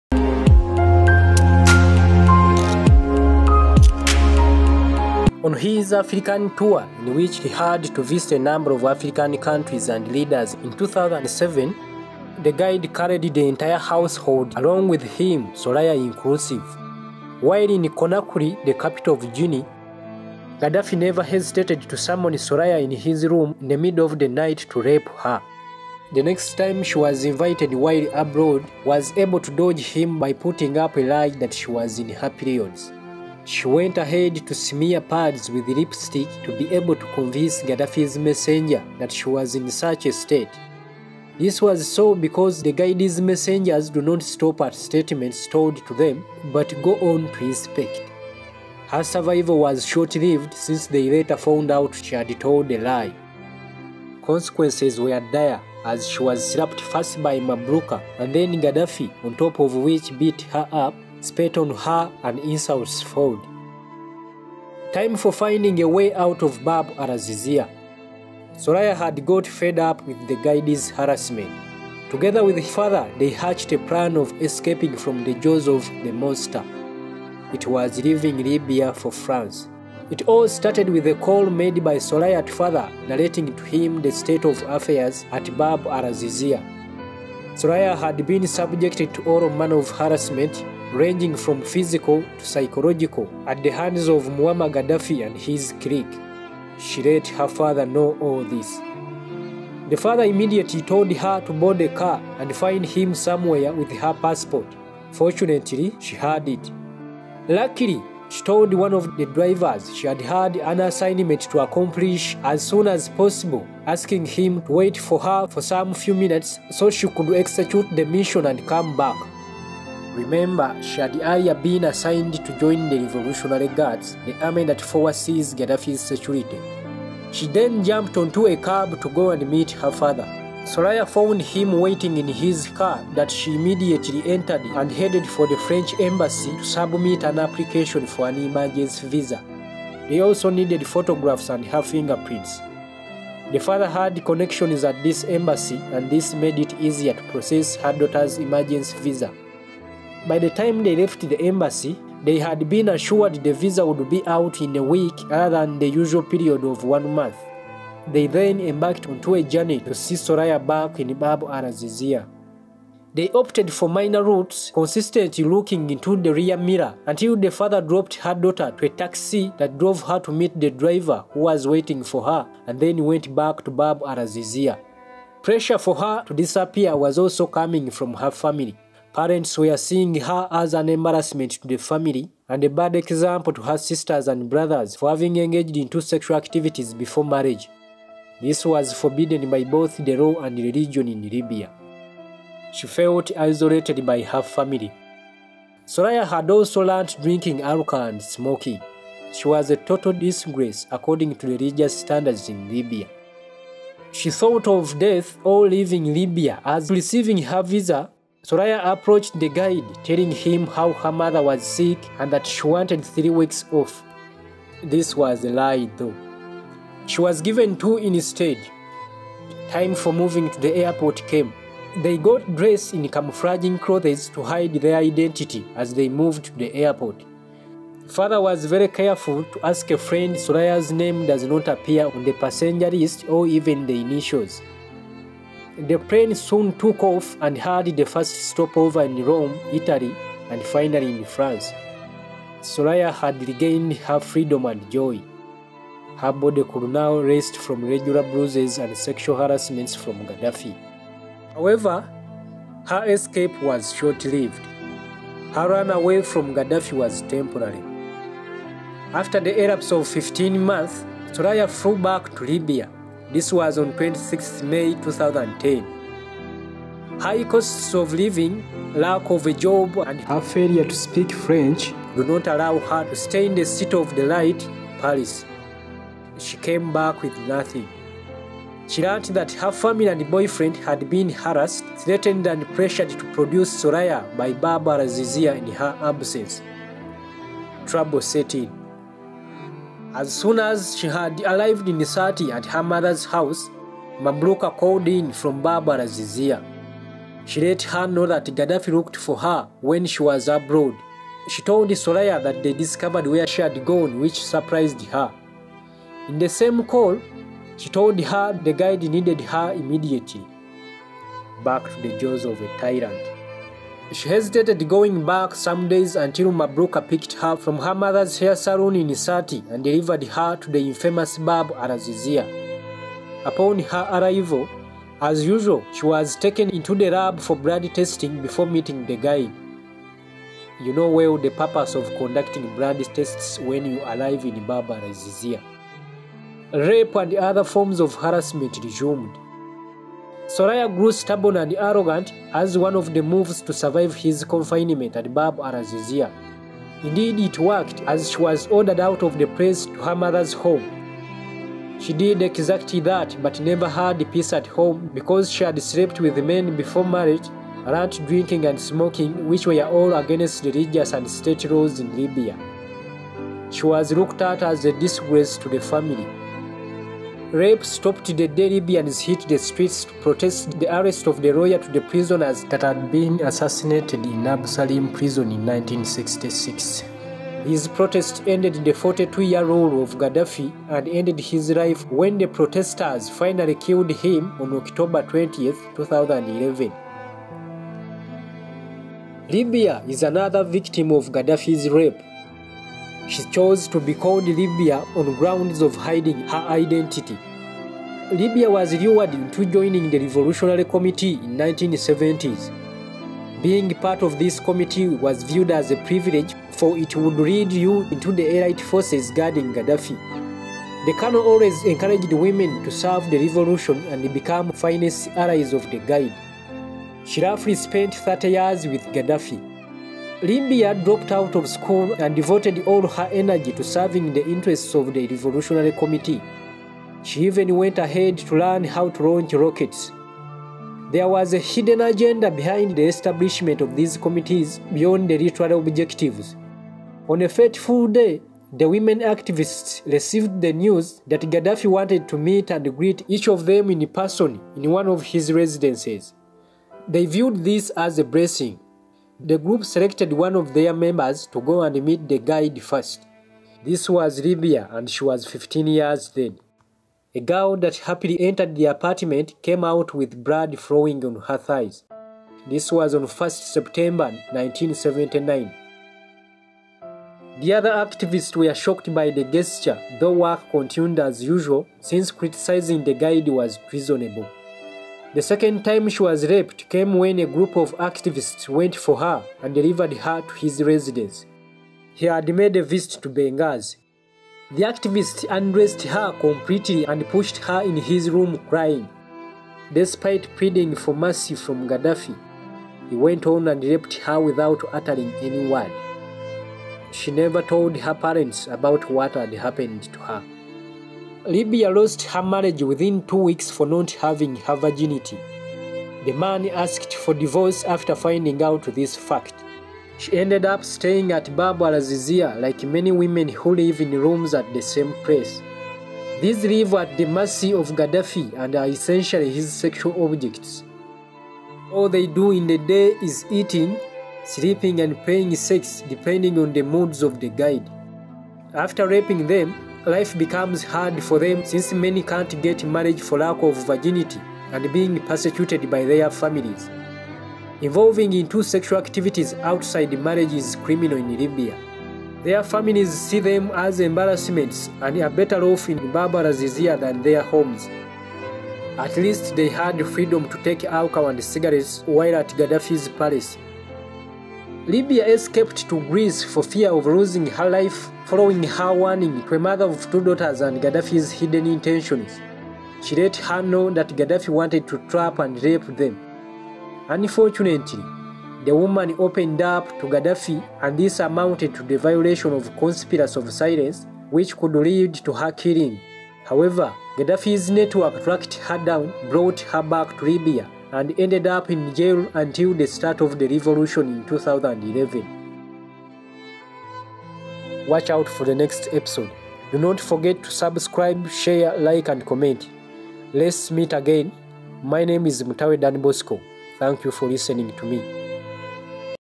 PART 3: The horrifying stories of girls raped by Gaddafi!
A number of girls are accusing the late col. Muammer Gaddafi of raping them . these say that he mostly did so in in his fortified home , Bab-Al -Azizia in Tripoli. Gaddafi mostly targeted virgin girls . He had a network that used to fetch them for him from schools and other places . Tap the link to enjoy the entire story ;